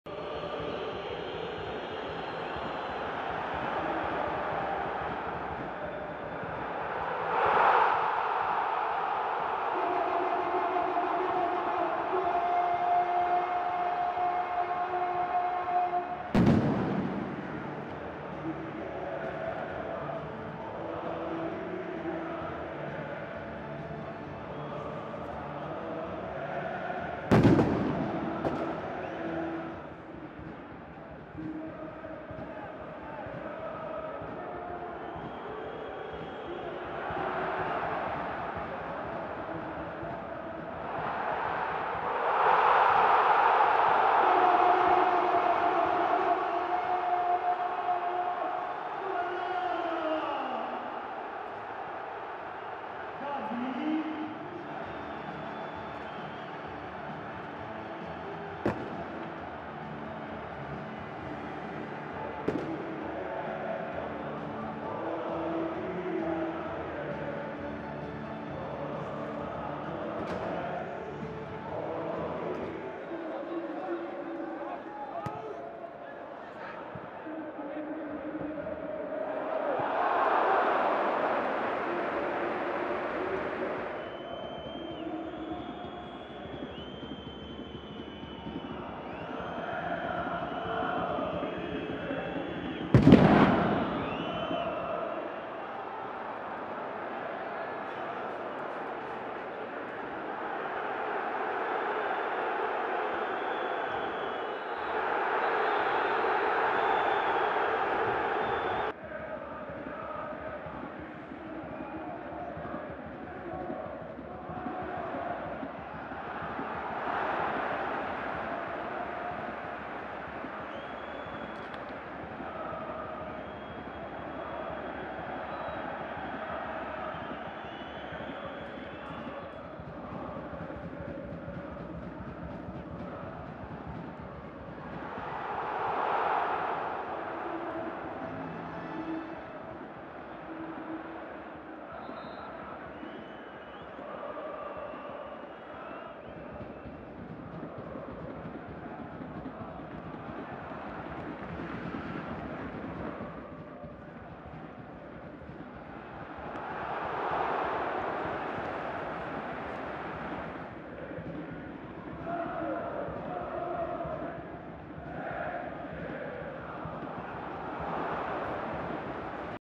Highlights: Ολυμπιακός - ΠΑΟΚ 2-1 / Highlights: Olympiacos - PAOK 2-1
Δείτε τα highlights από την αναμέτρηση του Κυπέλλου Ελλάδος ανάμεσα σε Ολυμπιακό και ΠΑΟΚ!